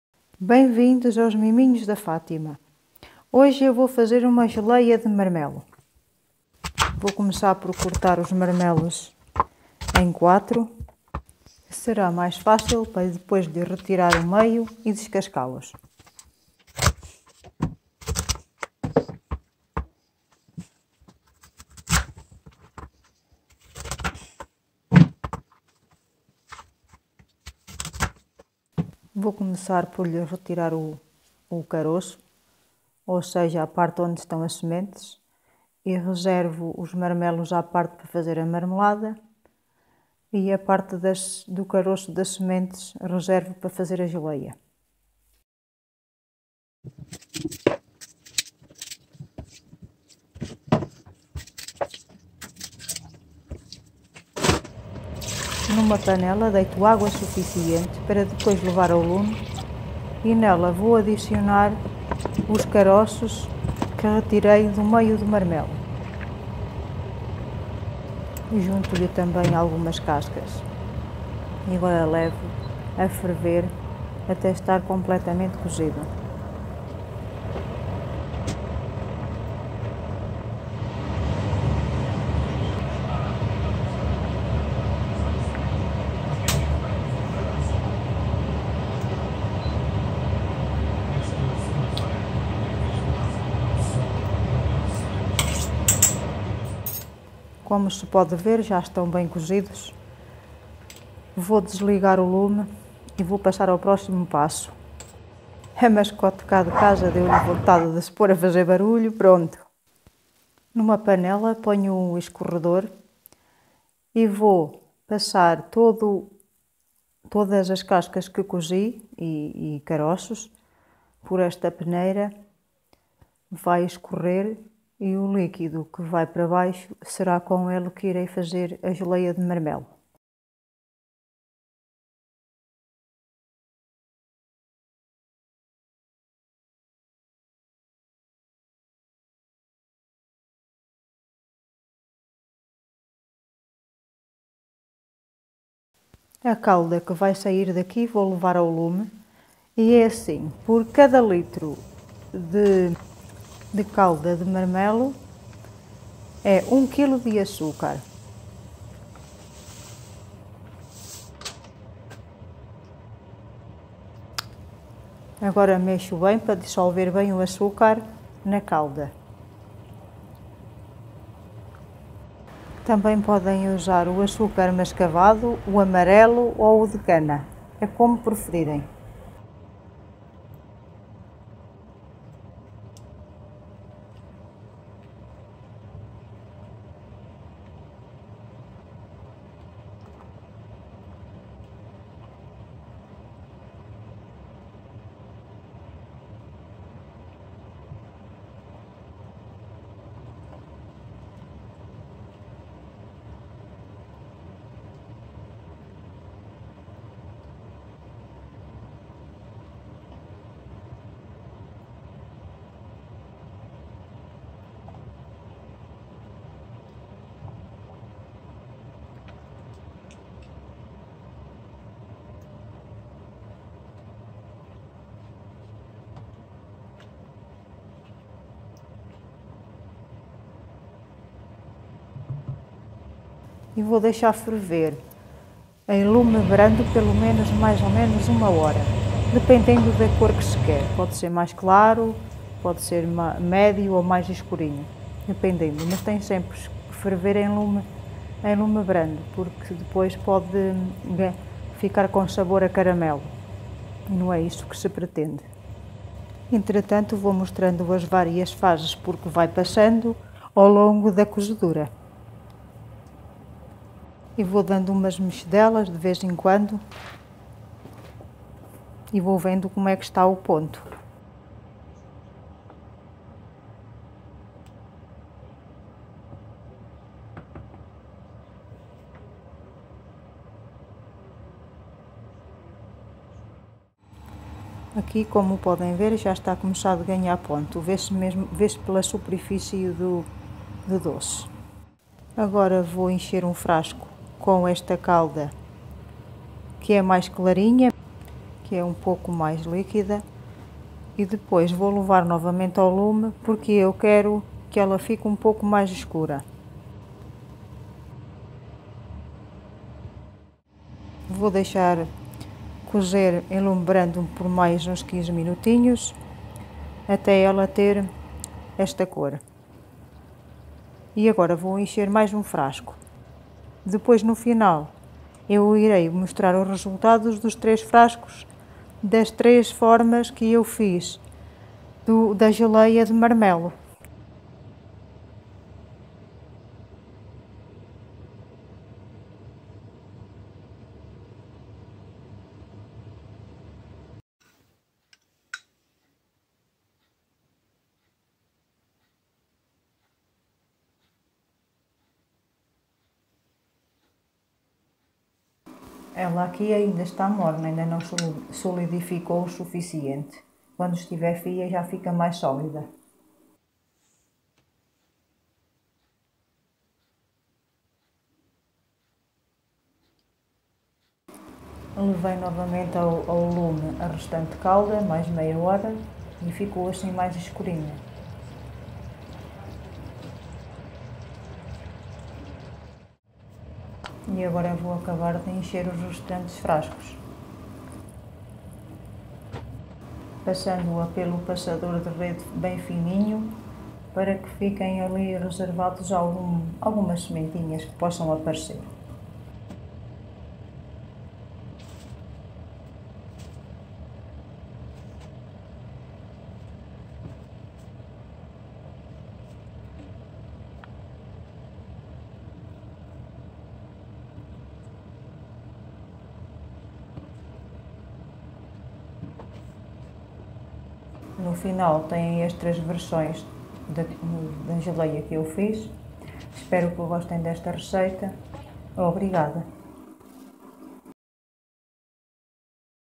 VOU FAZER UMA DELICIOSA GELEIA DE MARMELOS
marmelos
por cada litro de liquido da cozedura das cascas e caroços vai 1k de açúcar